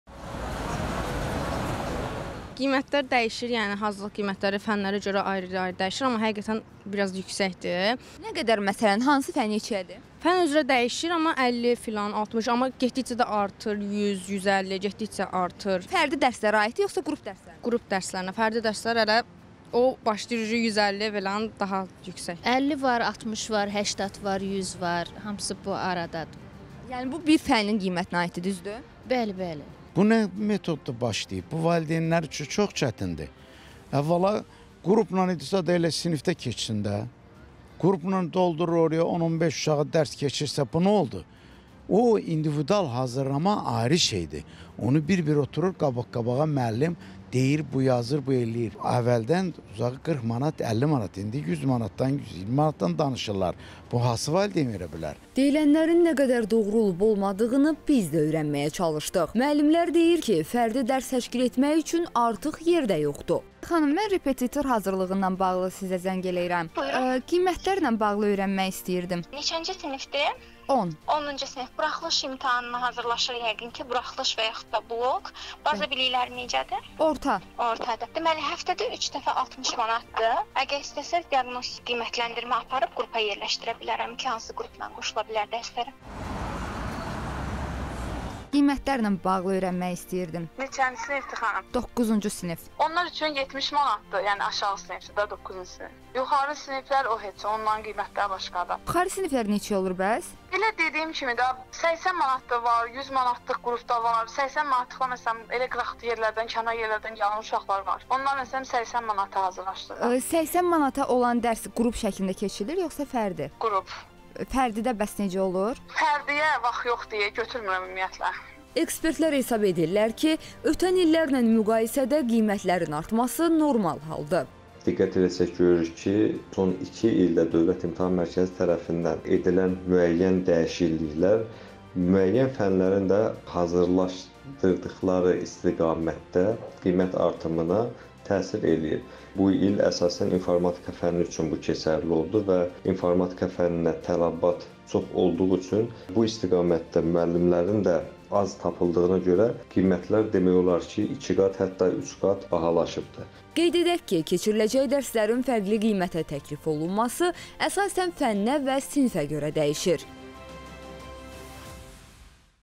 Valideynlər hazırlıq qiymətlərindən şikayətləndi: müəllimlər isə boş yer yoxdur deyir
Valideynlər dərsdən əlavə hazırlıqların qiymətlərinin baha olmasından narazıdırlar.
“Fərdi dərsləri 100-150, qrup dərsləri isə 50-60 manatdır”, – deyə valideynlər qeyd ediblər.
Məsələ ilə bağlı repetitorluqla məşğul olan müəllimlər isə bildiriblər ki, artıq fərdi hazırlıq üçün yer yoxdur.